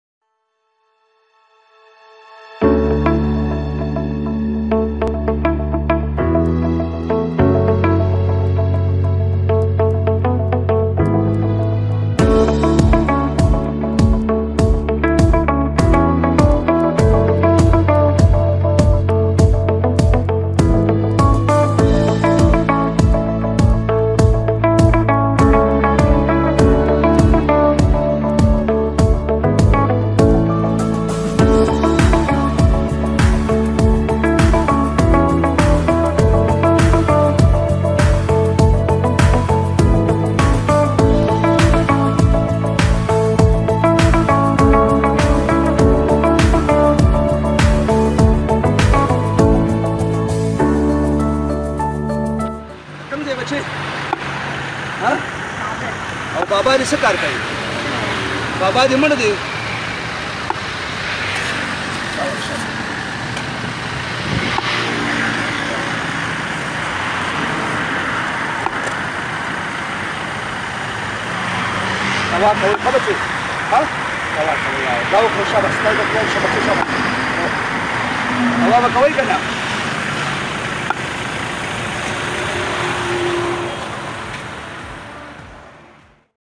peshawar traffic police officer Asif Raza helps children
یتیم بچے جو سخت گرمی میں مزدوری کرتے ہیں اور گھروالوں کا پیٹ پالتے ہیں خود صرف روٹی پر گزارہ کرتے ہیں پلیز آپ بھی مدد کر سکتے ھو۔